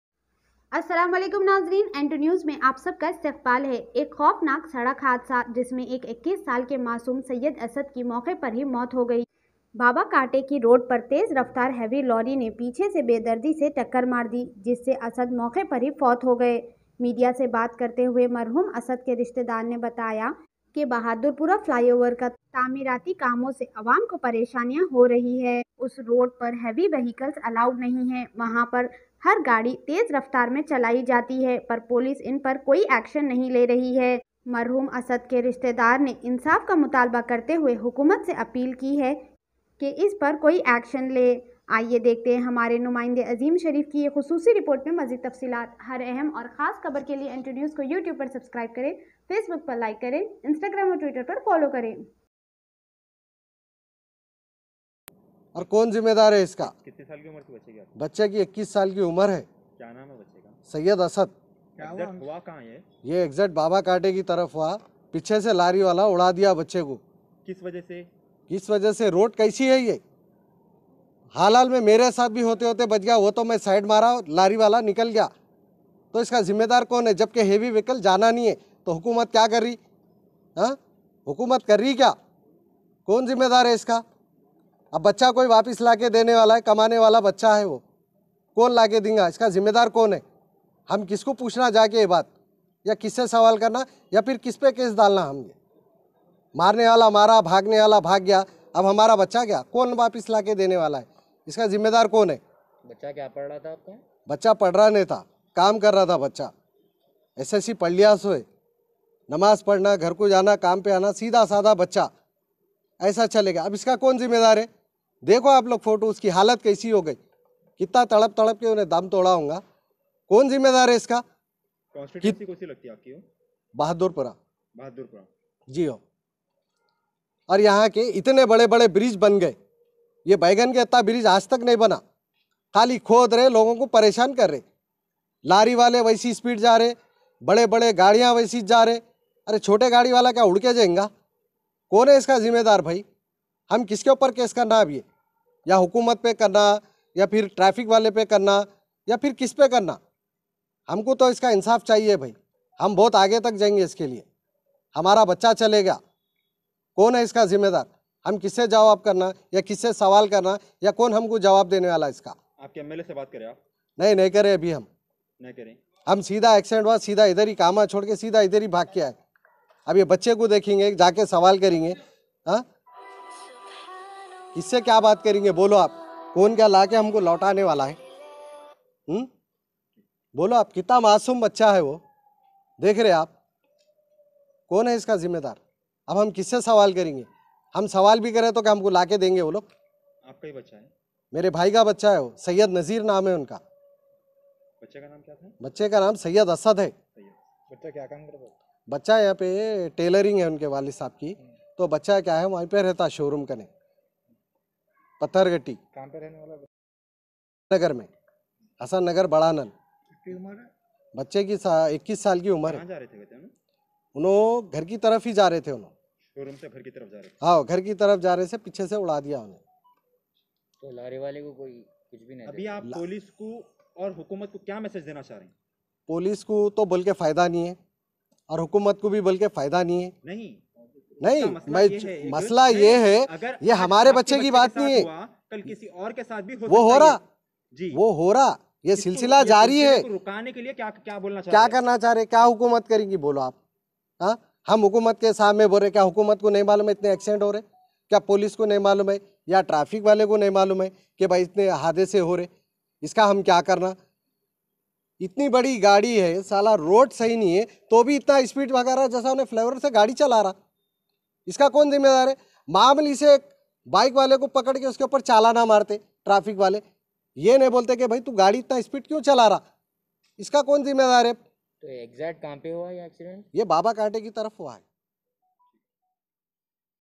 EK KHAUFNAAK SADAK HADESA PESH AAYA JAHA 21 SAALA BACCHE KO EK LORRY DRIVER NE PEECHE SE BEDARDI SE
EK KHAUFNAAK SADAK HADESA PESH AAYA JAHA 21 SAALA BACCHE KO EK LORRY DRIVER NE PEECHE SE BEDARDI SE MAARTE HI WO BACCHE KI MA?T Hogayi   NAZREEN JAB BHADURPURA ROAD PER HEAVY VEHICLES ALLOWED NAHI HAI WAHA PER HAR GADI TEZ RAFTAAR   ME JI CHALYI JAATI HAI SIRF POLICE KA KAAM CHALLAN MARNA HAI YA LOGO KI JAAN BACHANA HAI AAIYE DEKHTE HAI TAFSEELI REPORT